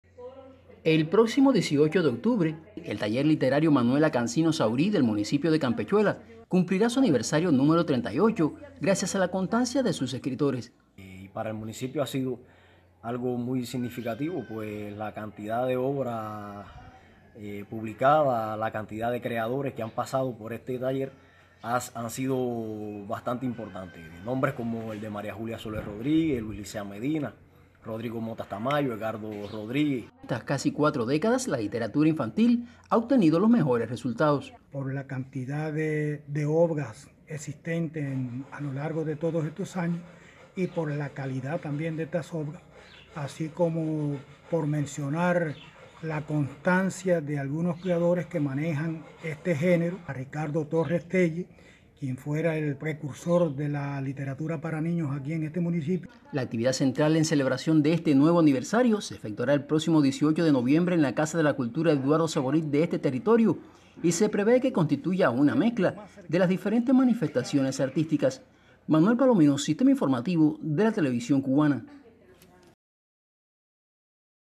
Taller Literario celebrará su aniversario 38 de creado
El taller literario Manuela Cansino Saurí, del municipio de Campechuela, celebrará este 18 de octubre su Aniversario 38. La constancia y calidad de sus creadores caracterizan sus casi 4 décadas de existencia.